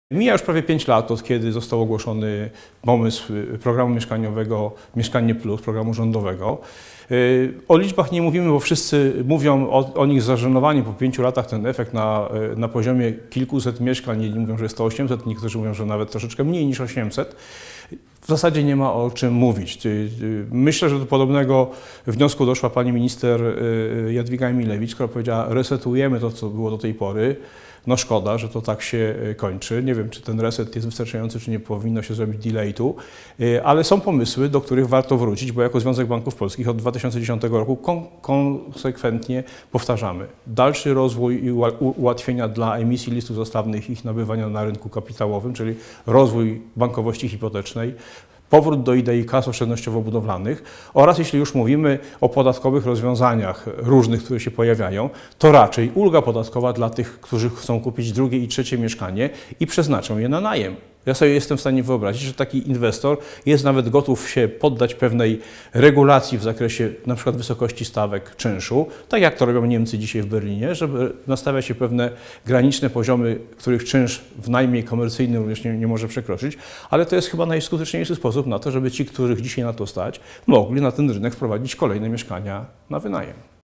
Rządowy program mieszkaniowy do kasacji, potrzebna jest ulga podatkowa
W kwietniu po raz pierwszy od siedmiu lat spadły nasze wynagrodzenia realne. Bezrobocie ma wzrosnąć do dwucyfrowego. Popyt na kredyty mieszkaniowe spada. Coraz bardziej brakować będzie mieszkań dla mniej zamożnych rodzin.

Mija już pięć lat odkąd ogłoszony został rządowy Program Mieszkanie Plus.

- O efektach wszyscy wypowiadają się z zażenowaniem, bo to zaledwie kilkaset mieszkań – mówi w rozmowie z MarketNews24 dr Jacek Furga, prezes AMRON. – Należy wątpić czy rządowi uda się reset tego programu.

Na rynku potrzebna jest większa liczba mieszkań na wynajem. Natomiast rola rządu nie powinna polegać na ich budowaniu

- Potrzebna jest ulga podatkowa dla kupujących mieszkania na wynajem, nawet pod takim warunkiem, że właściciel zgodzi się poddać regulacji dotyczącej stawek czynszu, tak jak to obecnie Niemcy robią w Berlinie – ocenia J.Furga.